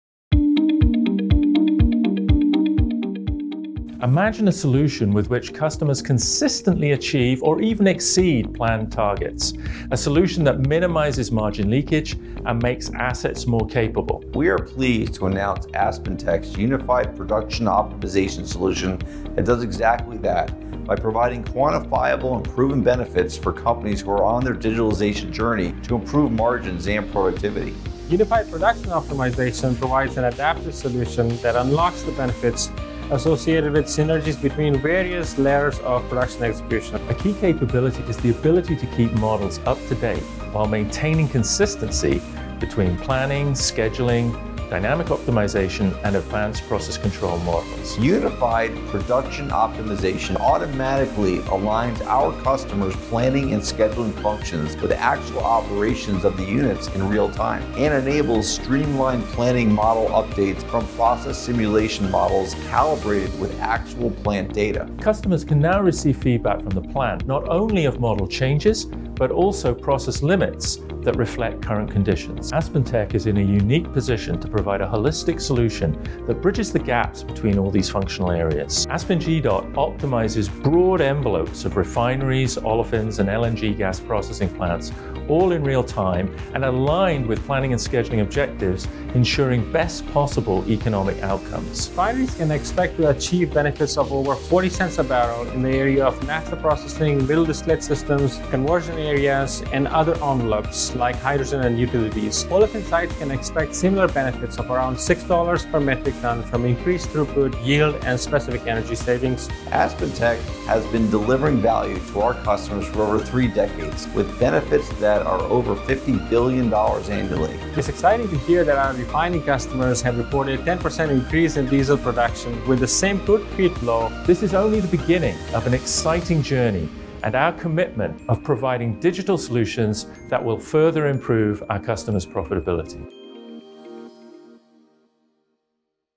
Now Available with aspenONE V11 - Unified Production Optimization
AspenTech’s Unified Production Optimization solution provides quantifiable benefits for companies who are on their digitalization journey to improve margins, increase productivity and adapt faster to changing marketing demands.